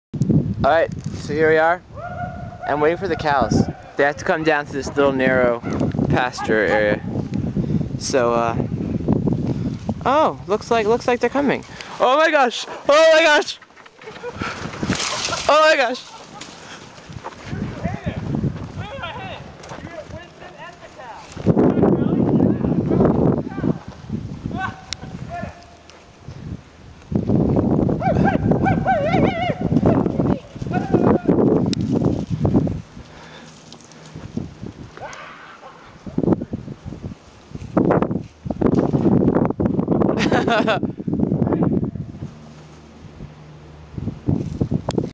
Wild-Cow-Boys
Yeah...Matt's cow + us bored = danger
and some good 'ole Carroll County fun : )